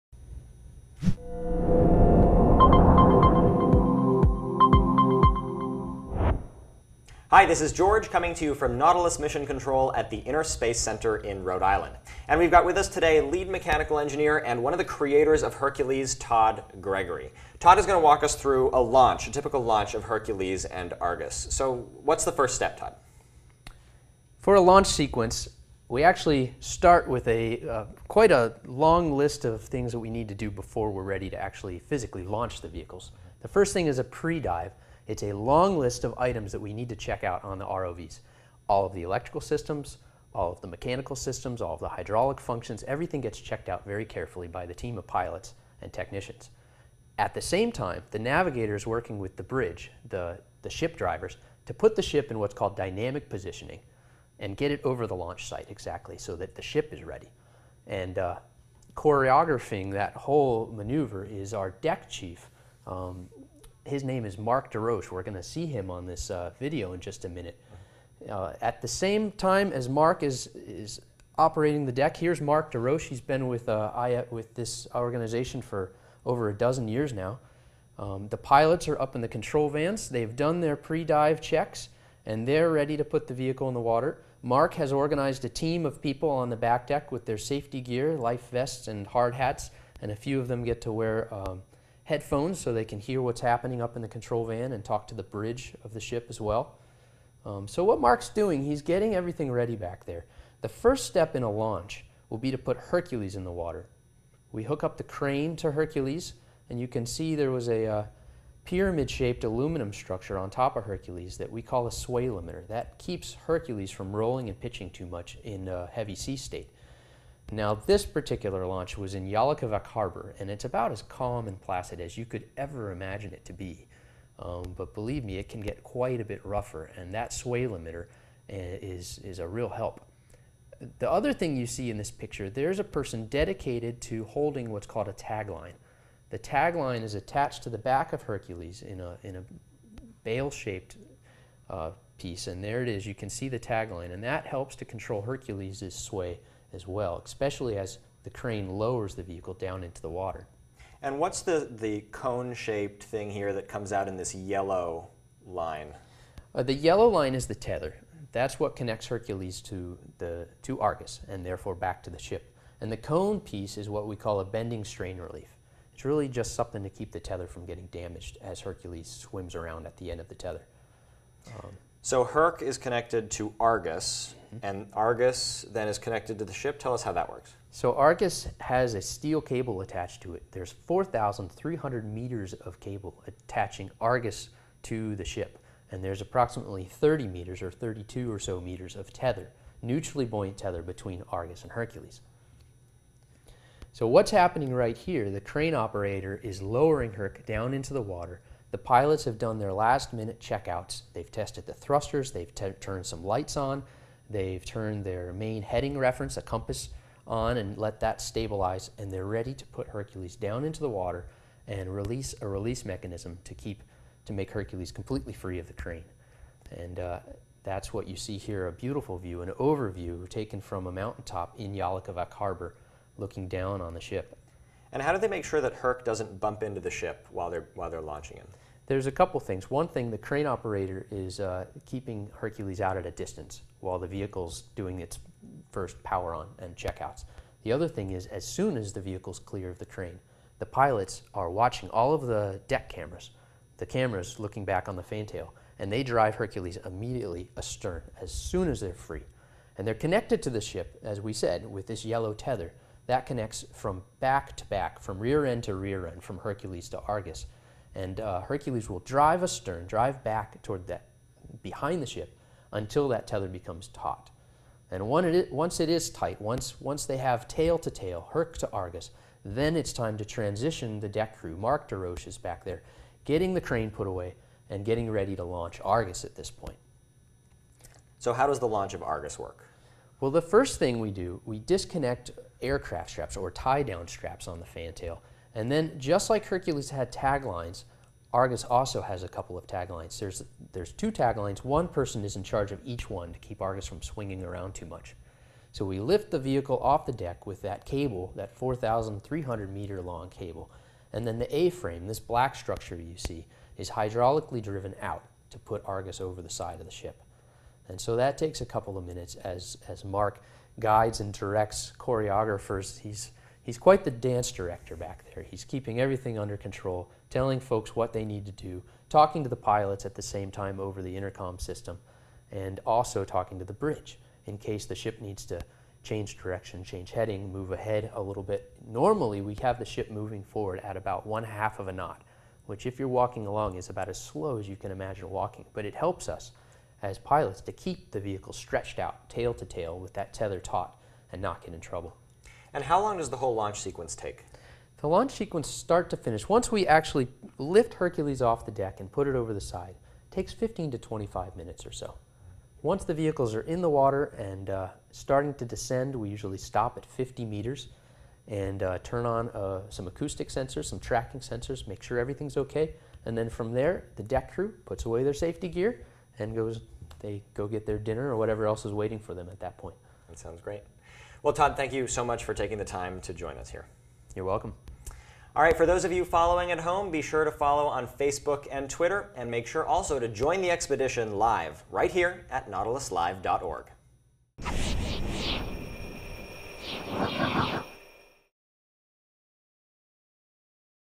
BEHIND THE SCIENCE 2012 | Launching ROVs
Explore the science behind launching ROVs: learn how the deck team, control van, and bridge all work together to get Hercules and Argus in the water.